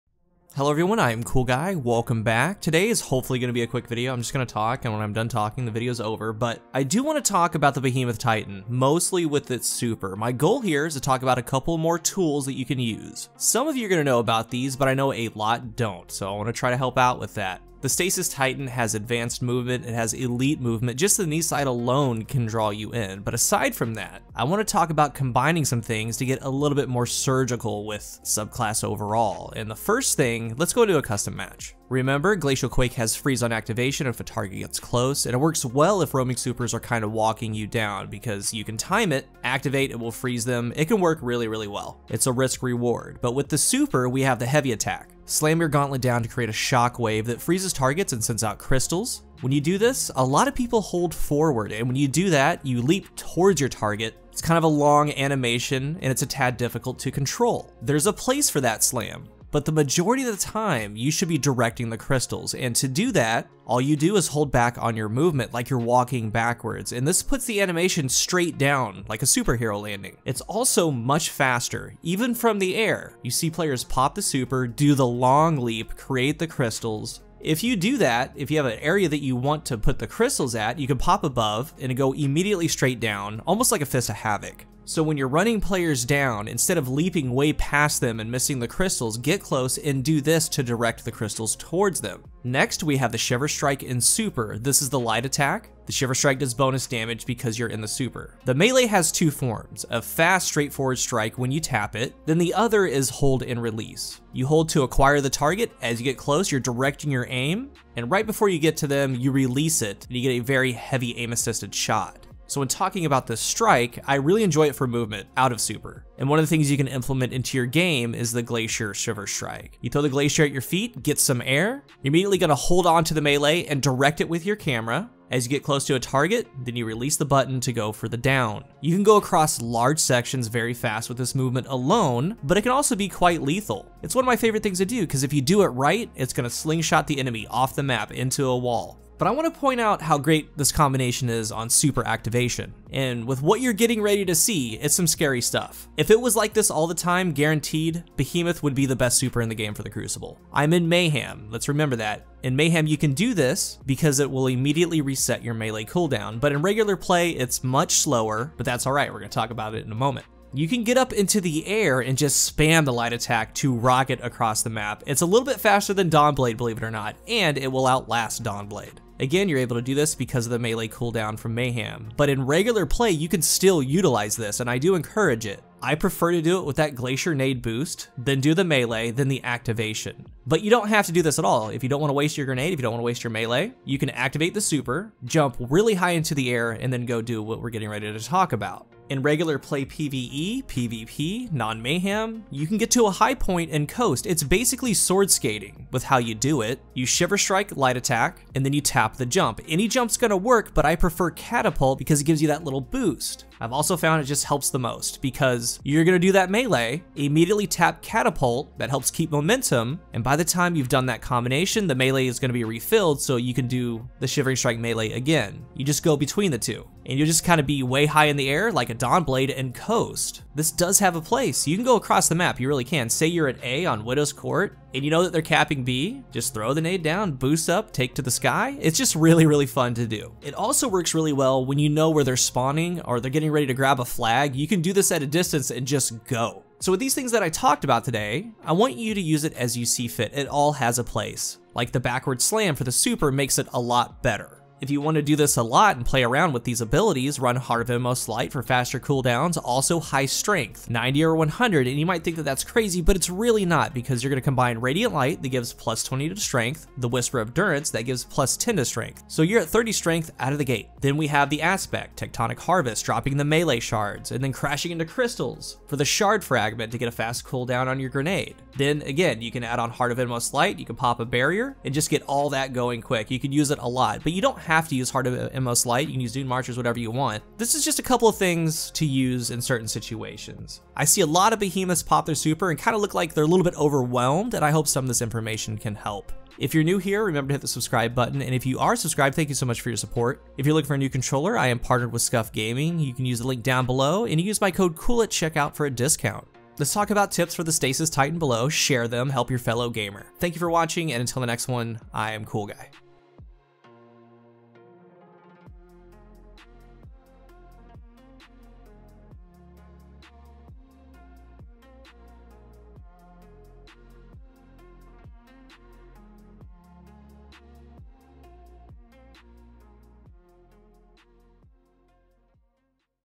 3 MUST KNOW Titan Behemoth movement/super tips
Todays video is a quick one about Titan movement on the Stasis subclass. Behemoth has excellent movement. I give three tips to try and put in your arsenal. What if one of the best ways to use behemoth was from the air?